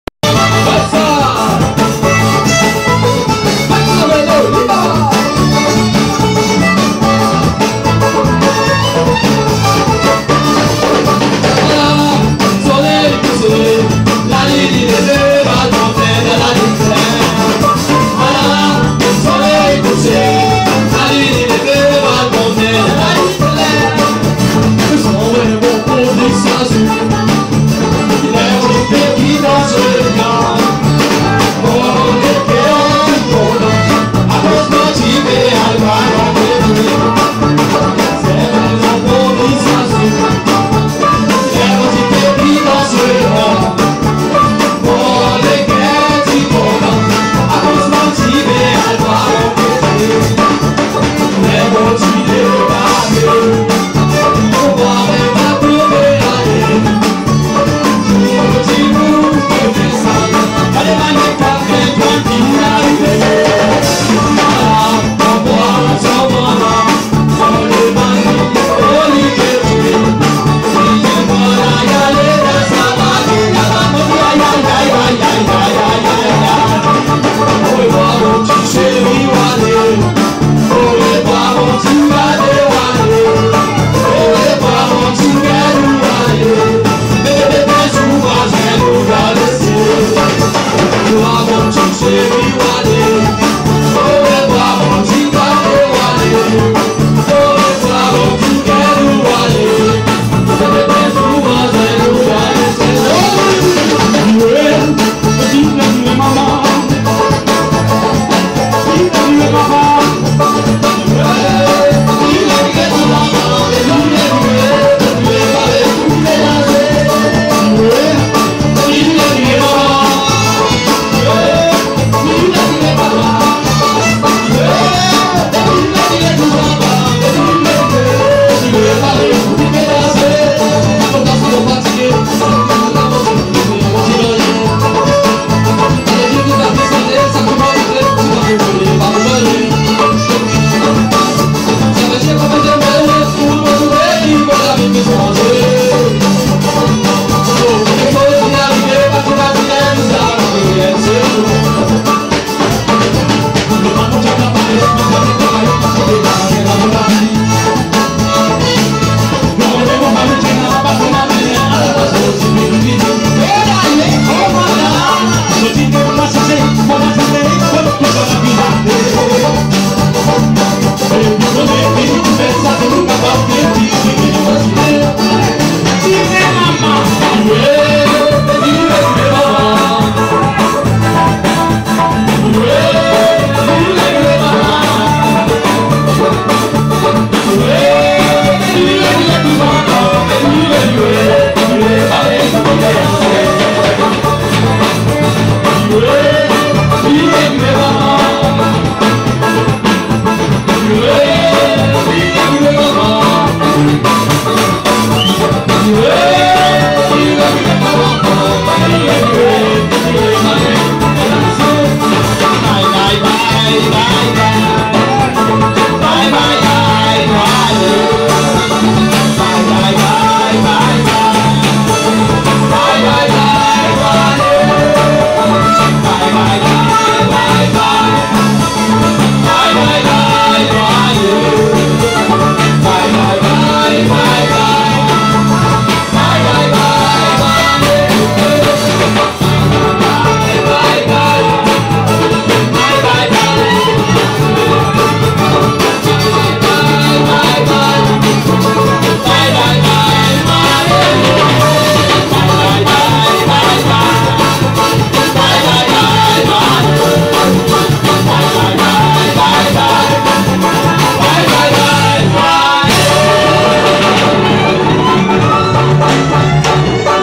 Seychelles dance 3, Festival kreol 2015, La Rue Bazaar
Festivale creole Seychelles Oct 2015, La Rue Bazaar